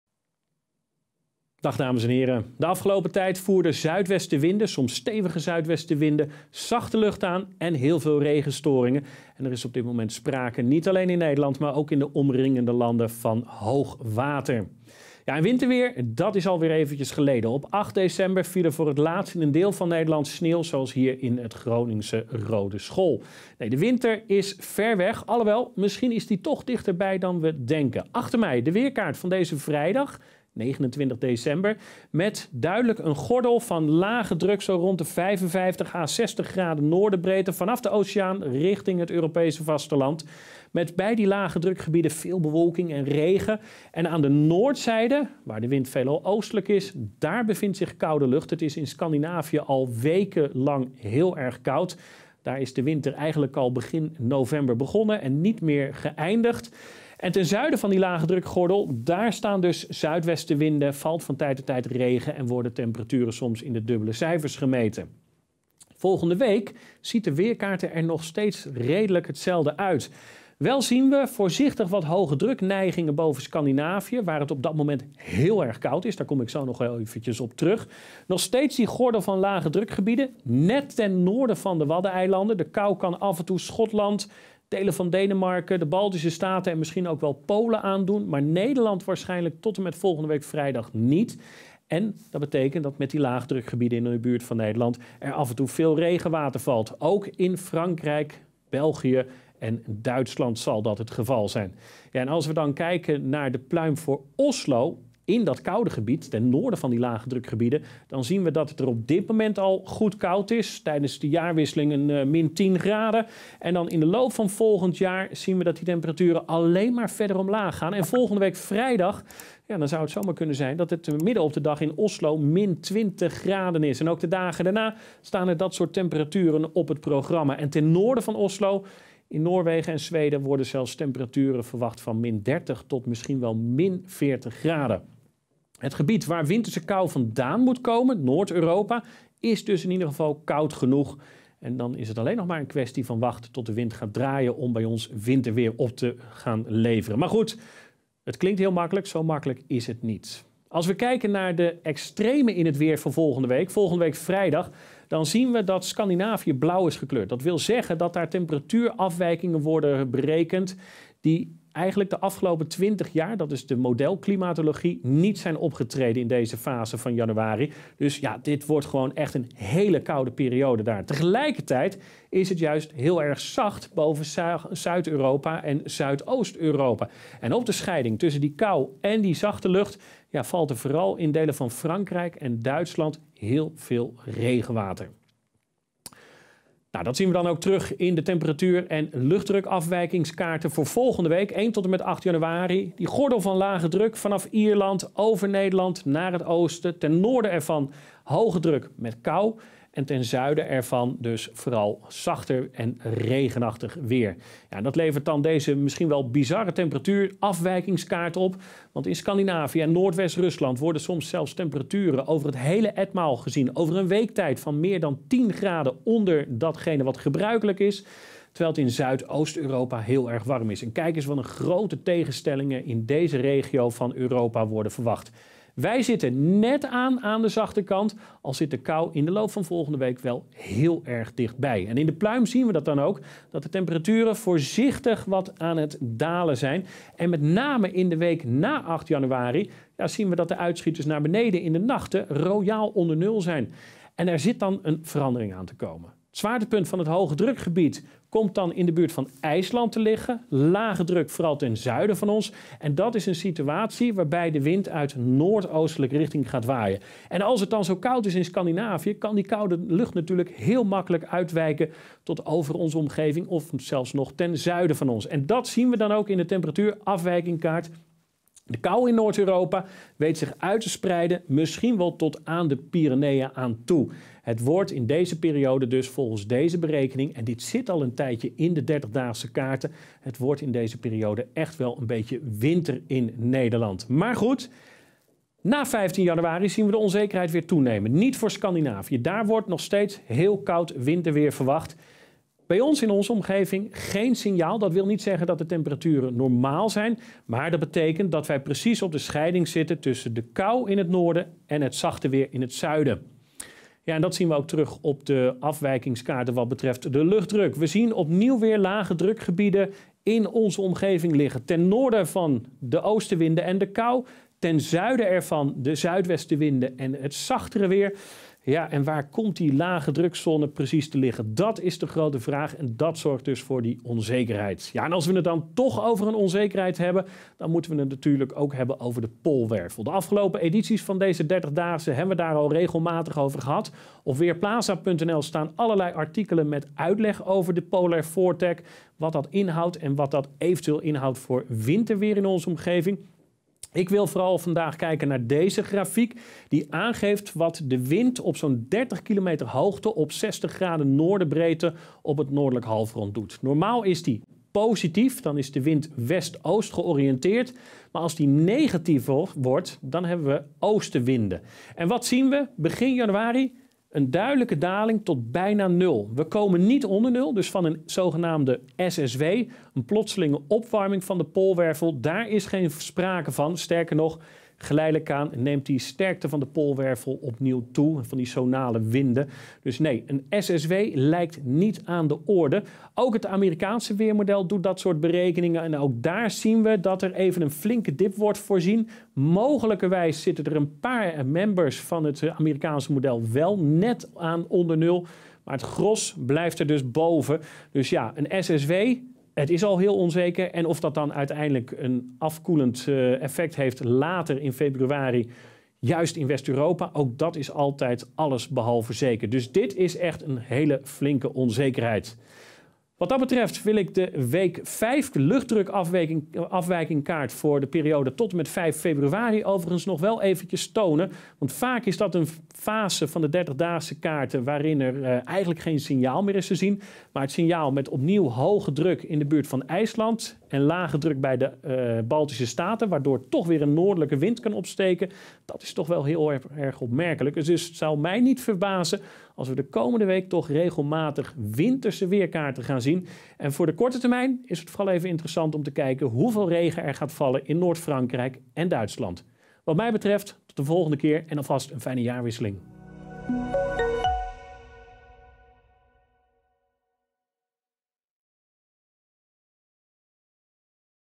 30-daagse: komt de winter uit zijn winterslaap? ❄️
Na een recordwarm en nat jaar, zien we begin januari veranderingen op de weerkaart verschijnen. Alfred Snoek vertelt je er meer over in een nieuwe 30-daagse!